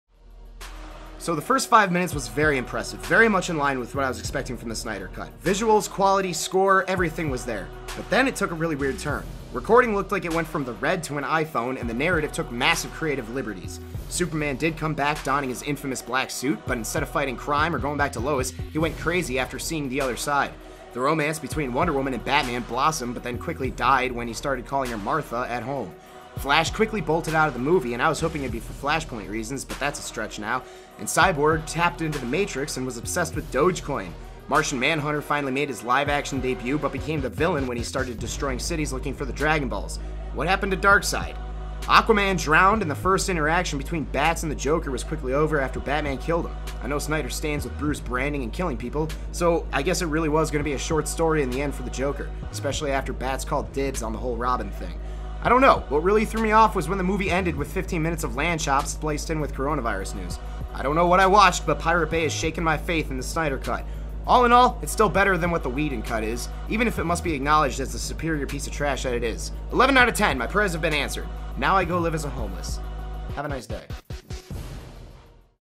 ASG Early Access Snyder Cut Just Ice League Review
With early access to the Snyder cut from pirate bay, I give my review of the unexpected. who would have thought the dragon balls make an appearance?!? Go watch the Snyder cut on the 18th and miss the joke until then.